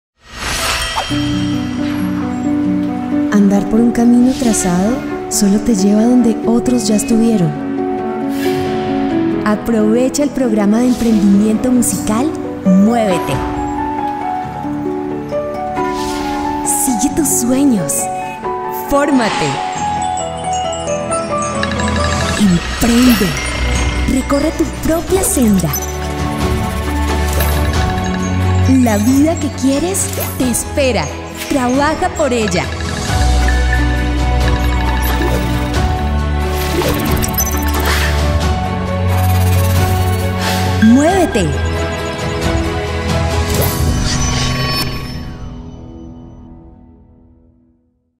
MUÉVETE - PROMO 06
Promocional de campaña de incentiva al talento juvenil.